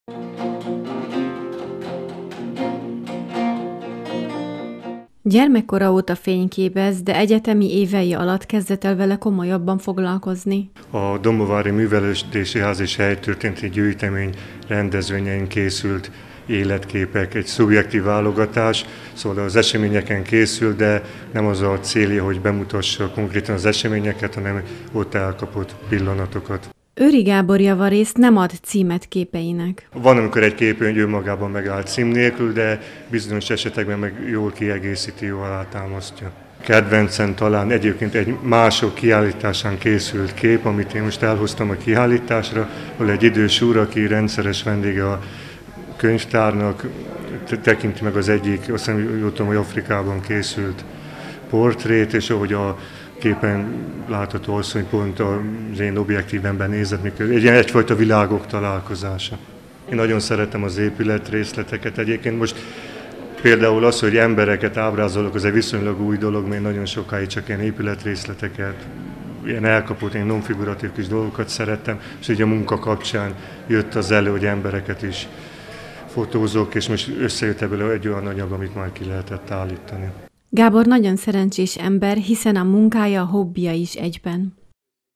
Emberek a házban - Őri Gábor fotóiból nyílt tárlat
Emberek a házban címmel nyílt Őri Gábornak fotókiállítása a Dombóvári Helytörténeti Gyűjteményben.